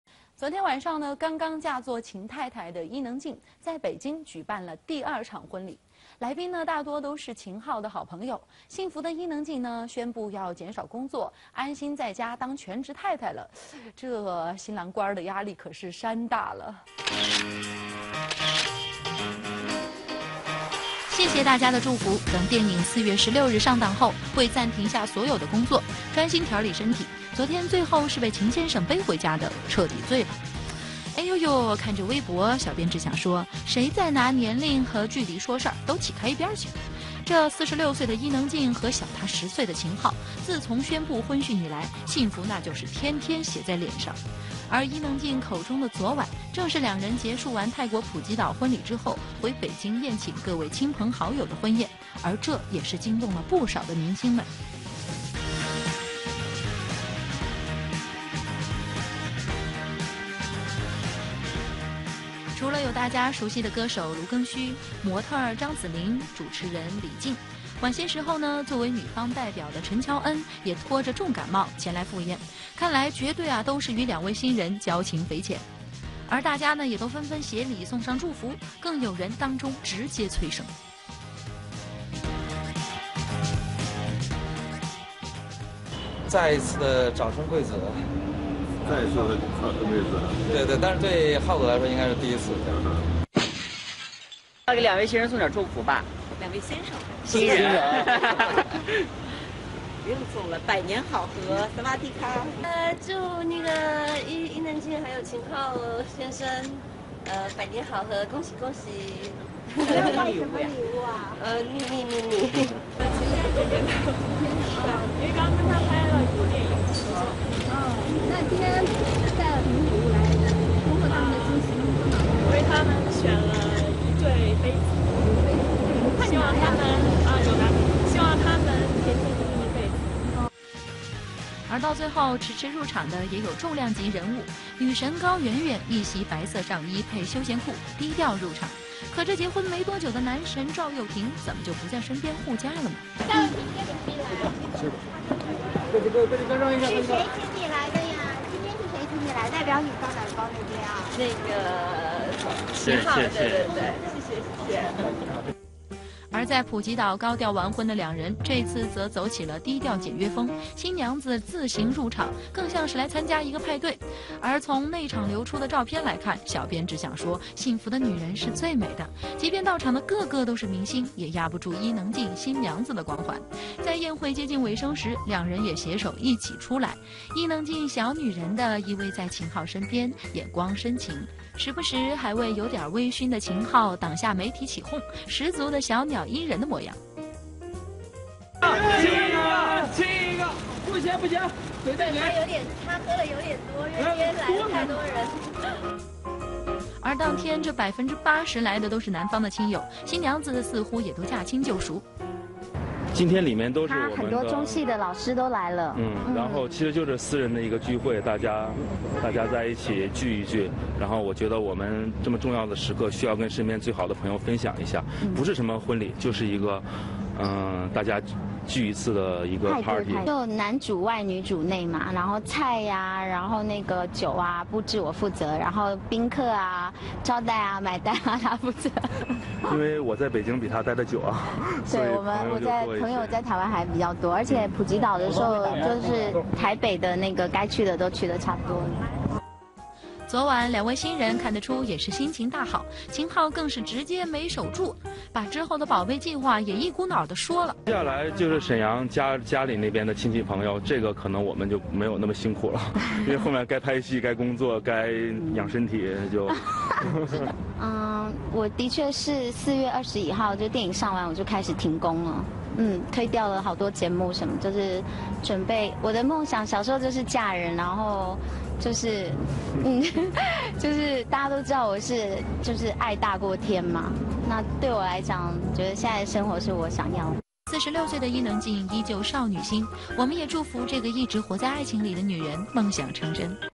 伊能静北京婚礼 伊能静婚宴图要修过却遭好友晒原图
46岁的伊能静[微博]嫁给小10岁的男星秦昊[微博]，两人在泰国普吉岛完婚，但她表示要“修图过后”才会公开婚礼现场照，也不淮参加宾客拍照上传至网络，2天后公开婚礼照，3月31日回到北京举行第二场婚宴，未料《时尚芭莎》总编辑苏芒开心地率先曝光现场照，意外让网友得知她要花时间P图的原因。